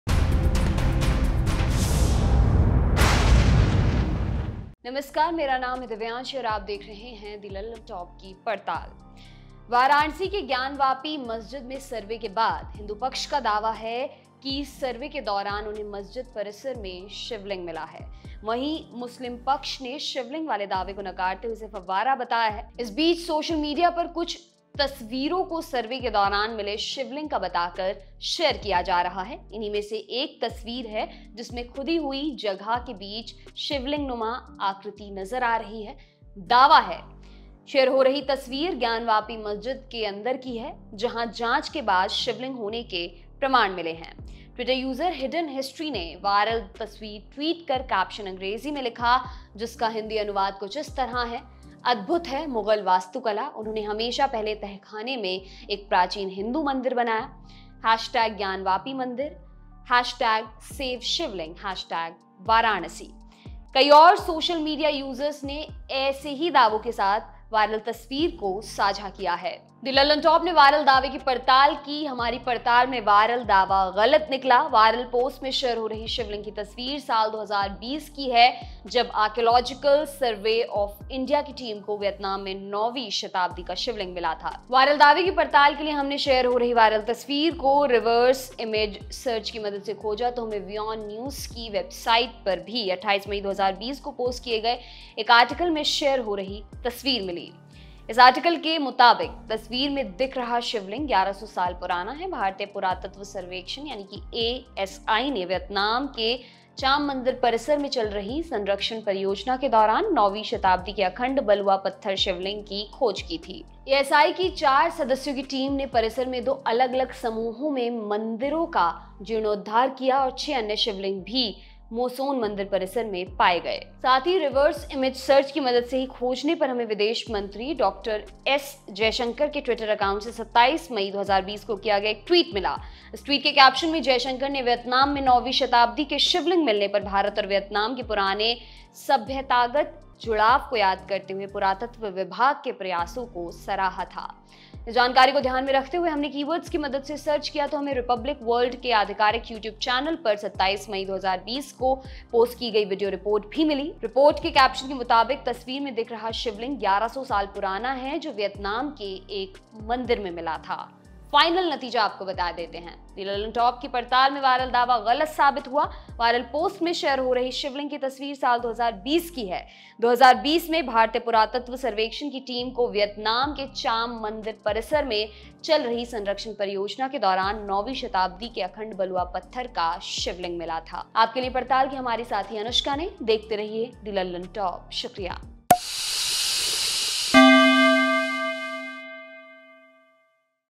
Gyanvapi Masjid में मिला Shivling बताकर वायरल फोटो का सच जान लीजिए |Gyanvapi Mosque News|Varanasi
Social media users are sharing a picture and claiming that the picture being shared is inside the Gyanvapi mosque where after investigation, evidence of Shivling has been found.
The picture of Shivling being shared in the viral post is of the year 2020. The team of Archaeological Survey of India (ASI) found a 9th century Shivling in Vietnam.